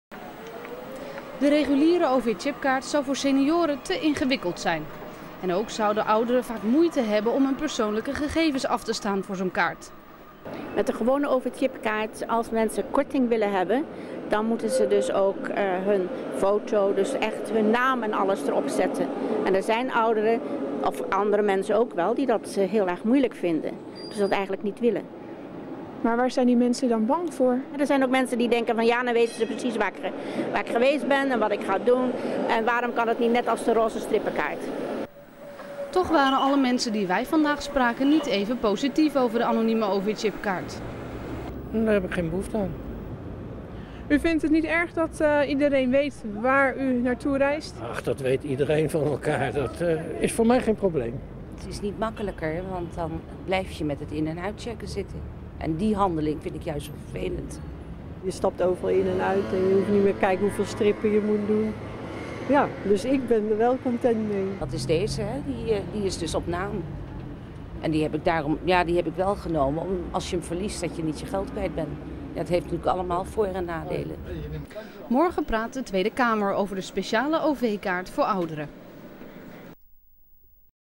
VVD en Groenlinks willen roze ov-chipkaart - Westonline.nl
REGIO - Regeringspartij VVD en oppositiepartij Groenlinks willen een roze ov-chipkaart met seniorenkorting, die een 65-plusser anoniem kan kopen.

Tweede Kamerlid Ineke van Gent van Groenlinks waarschuwt al langer dat ouderen het te ingewikkeld vinden en moeite hebben om hun persoonlijke gegevens door te geven om in aanmerking te komen voor de seniorenkorting bij de reguliere ov-chipkaart. Groenlinks en de VVD pleiten donderdag tijdens een Kamerdebat voor een 'losse' kortingskaart voor ouderen die ze anoniem kunnen kopen.

Volgens de partijen hoeven de vervoersbedrijven niet te vrezen voor fraude en dat geanonimiseerde kaarten met korting aan jongere familieleden overgedragen worden. Dat zou ondervangen kunnen worden door de losse ov-chipkaart voor senioren roze te maken, net als de strippenkaart voor 65-plussers.